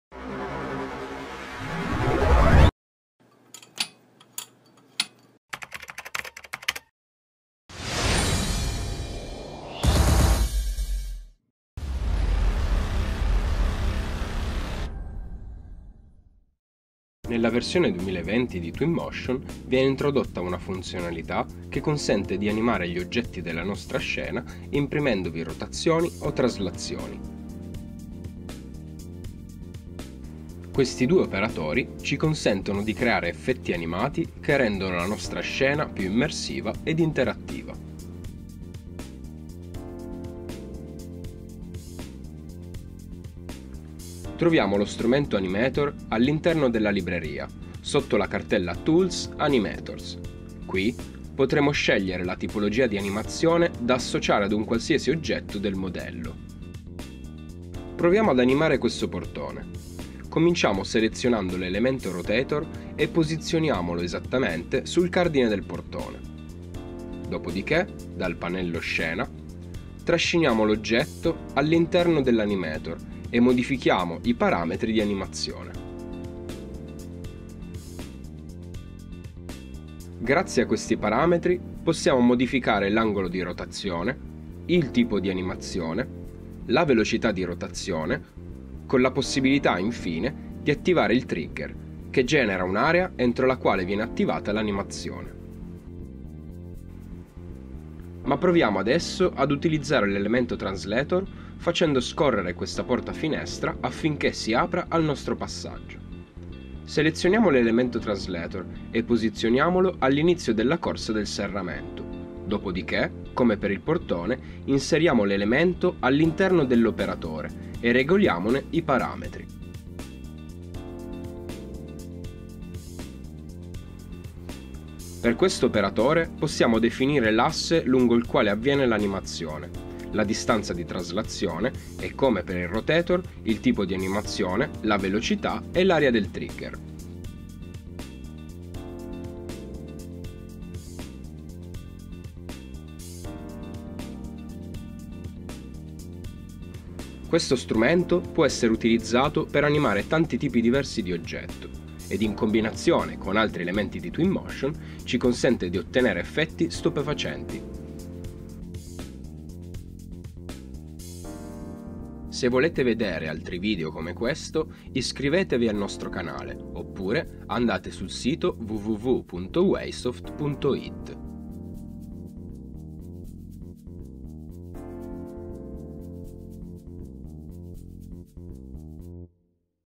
TWINMOTION 2021 PILLOLE | ANIMARE OGGETTI NELLA SCENA
TWINMOTION 2021 PILLOLE analizza argomenti specifici per aiutare gli utenti a perfezionare l'utilizzo del renderizzatore. Con ANIMARE OGGETTI NELLA SCENA ti mostriamo come dare vita a qualsiasi modello 3D importato in Twinmotion!

Twinmotion è un nuovo software di rendering 3D, ideato per aiutare il progettista a rappresentare i propri progetti con qualità e velocità, non solo immagini ad alta qualità ma anche vere e proprie esperienze con video VR interattivi e tour virtuali a 360° in 4k.